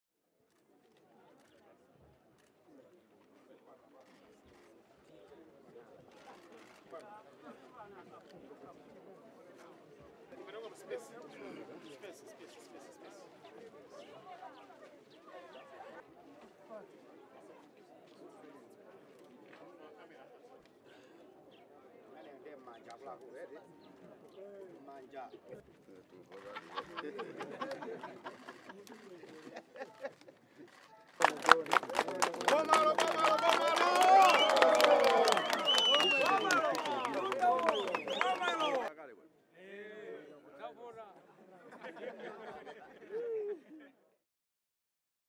Malawi Congress Party's Lazarus Chakwera votes in general election
Malawi Congress Party's candidate Lazarus Chakwera casts his vote for the country's presidential election in a rural polling station near his home village in Malawi's Lilongwe area. IMAGES